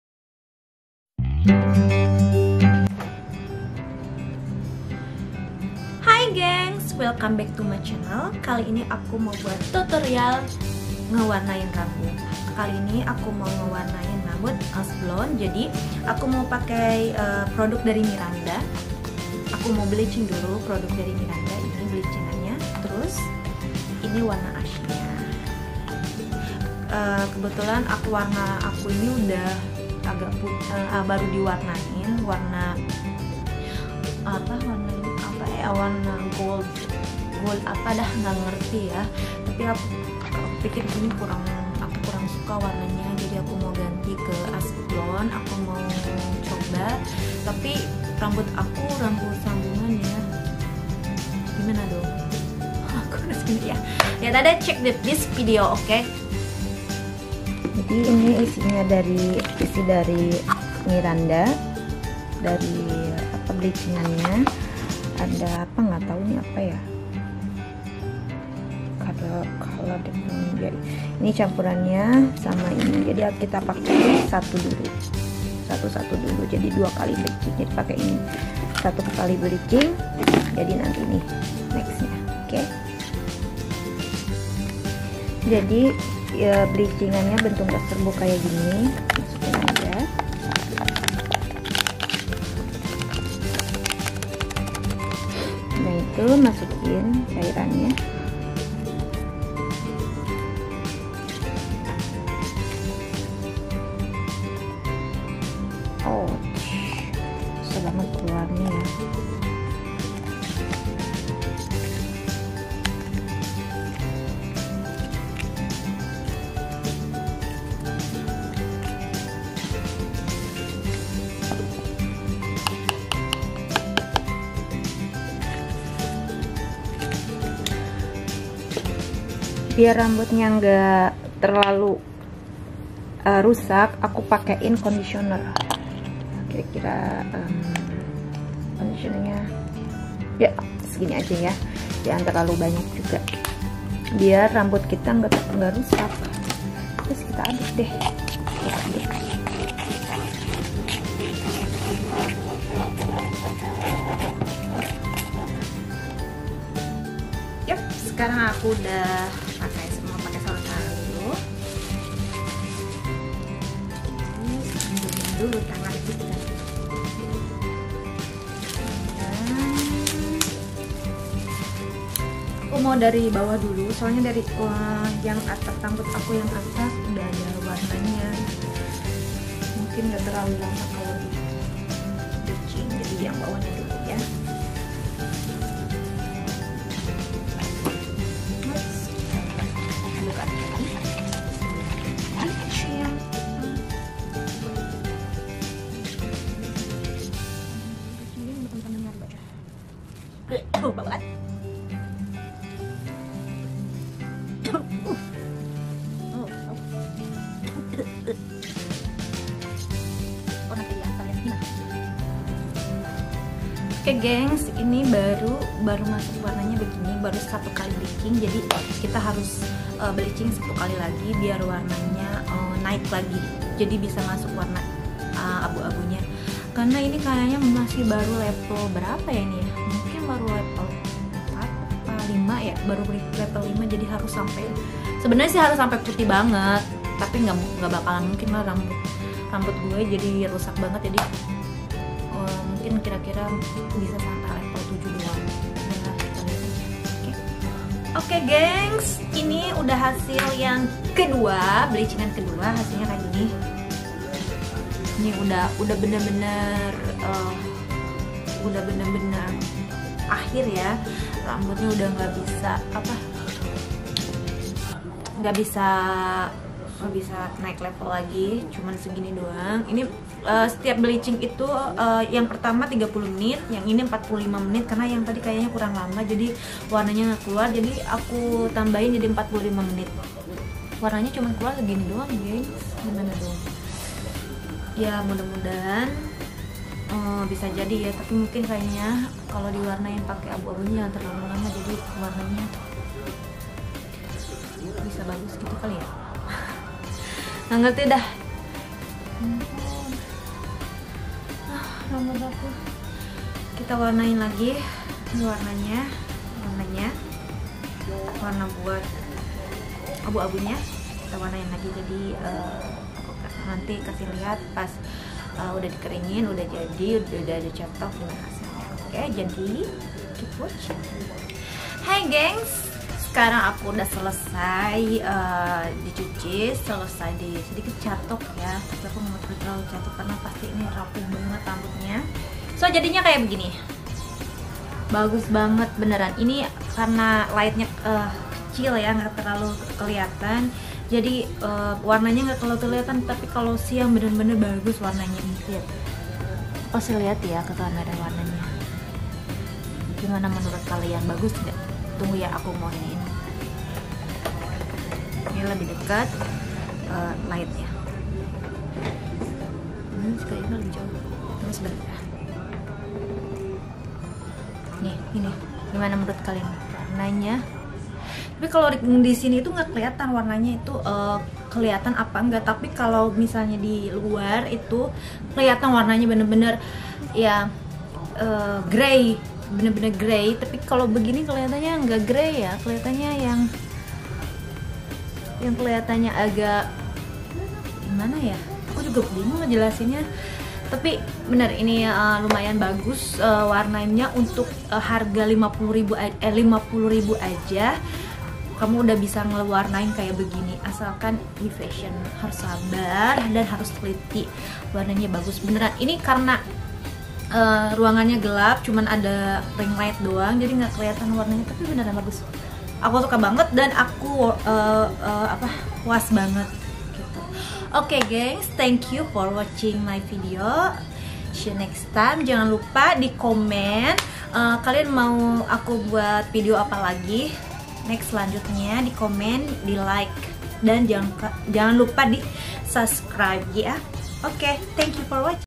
Cara mewarnain rambut ash blonde pakai Miranda
Hey , guys ini aku ingin share ke kalian gimana cara ngewarnain rambut pakai cat murah Miranda . Jangan lupa untuk campuran warna nya pakein conditioner untuk menjaga rambut ga begituh rusak .